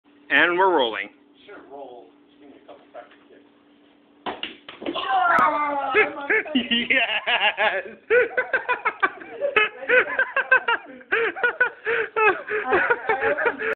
The Roommate Field Goal
When my roommates and I get bored we play sports. with ourselves as the goal!